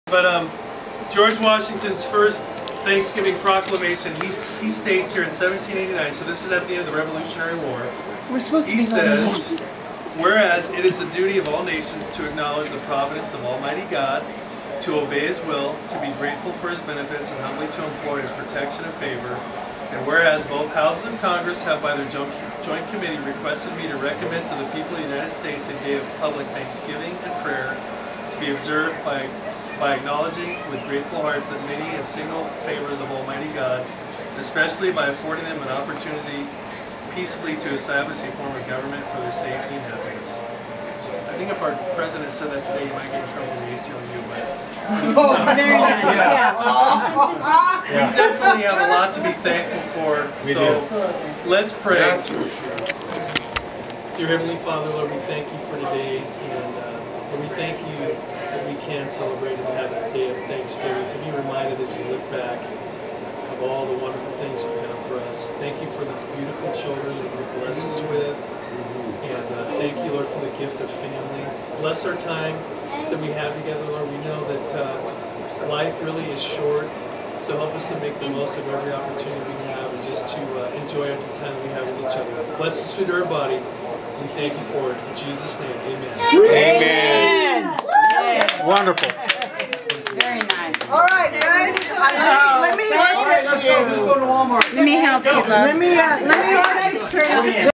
Kenny Stice telling the story of Thanksgiving and saying grace. this is not a great video, but you get to feel the love in the room.